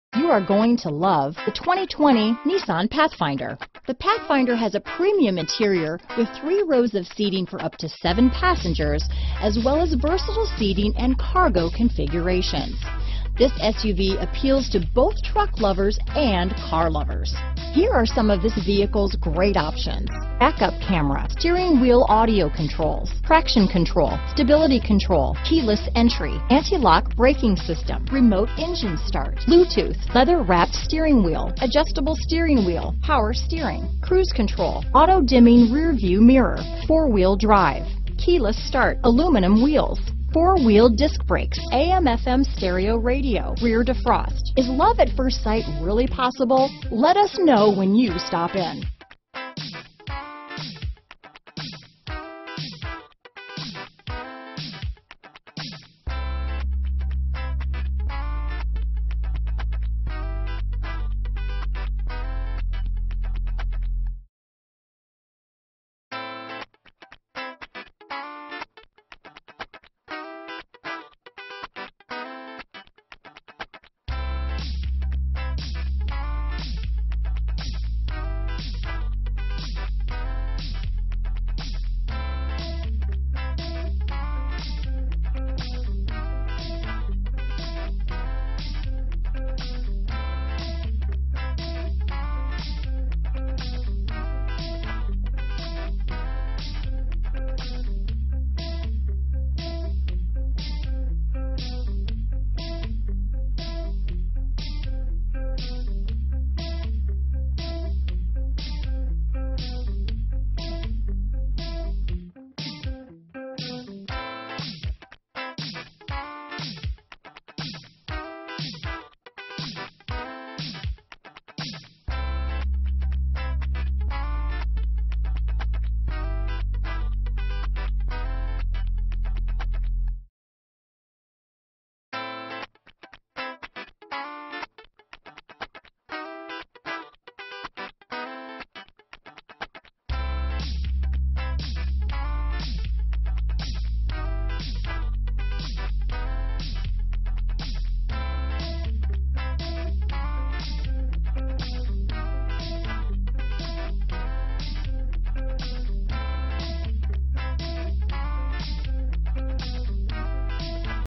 2020 Nissan Pathfinder Albany, Colonie, Latham, Johnstown, Glens Falls, NY N20P0939
Magnetic Black Pearl New 2020 Nissan Pathfinder available in Albany, New York at Destination Nissan. Servicing the Colonie, Latham, Johnstown, Glens Falls, NY area.

Destination Nissan
770 Central Ave